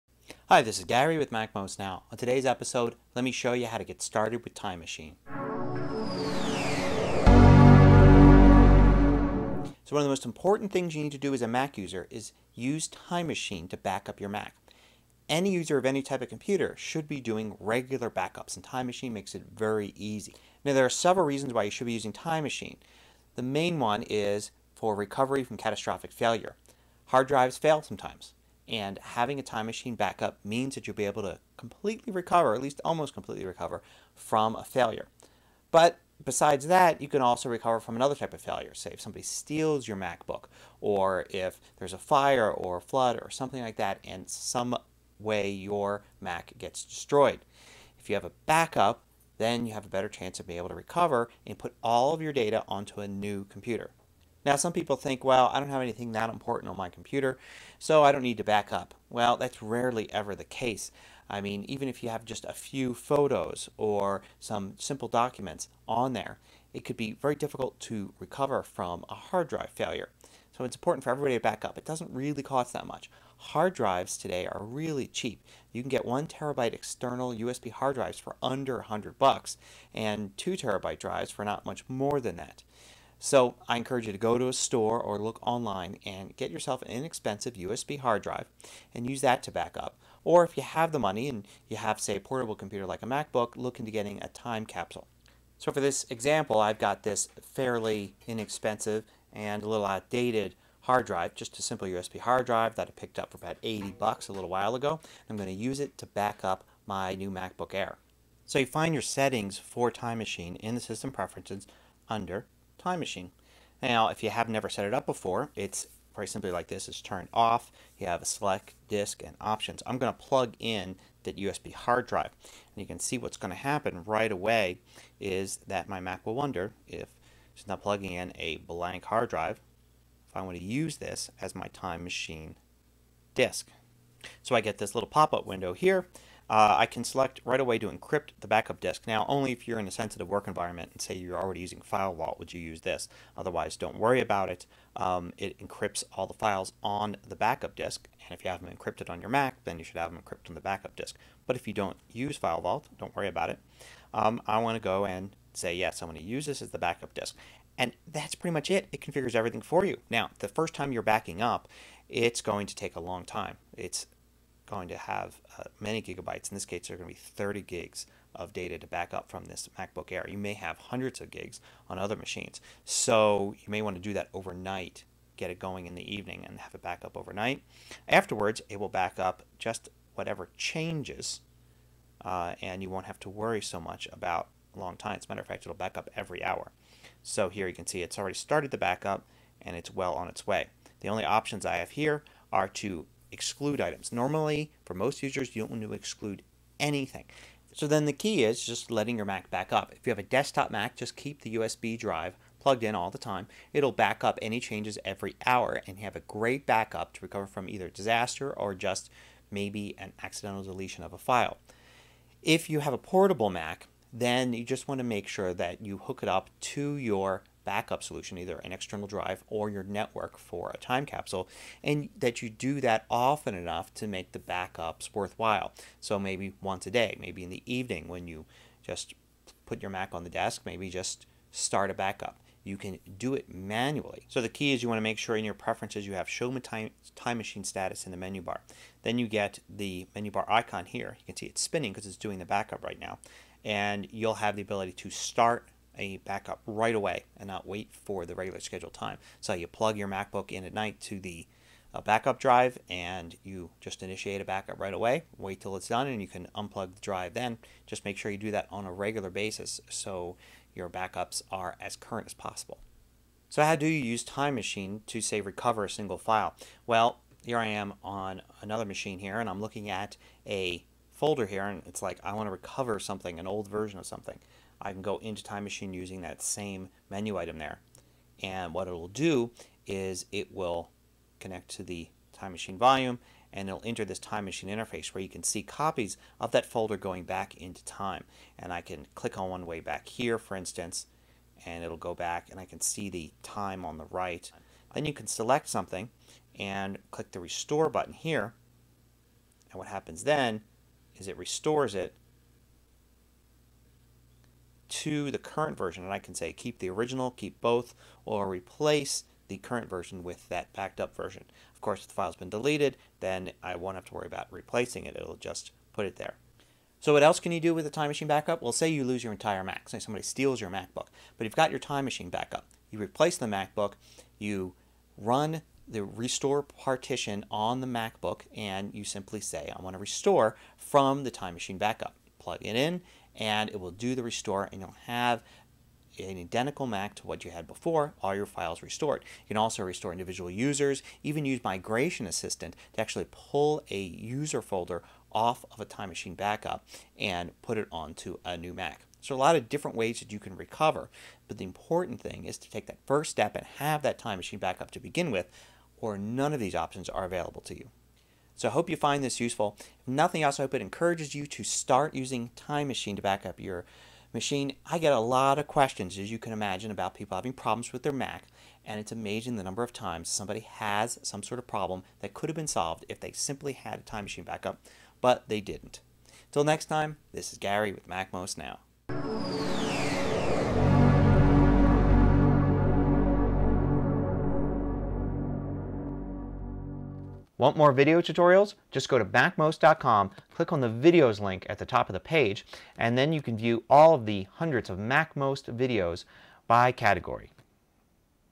Backing Up Your Mac With Time Machine (MacMost Now 603)
Learn the basic steps to start backing up your Mac with Time Machine. Every computer user should back up his or her data on a regular basis, and using Time Machine on your Mac makes that easy. Find out how to get started.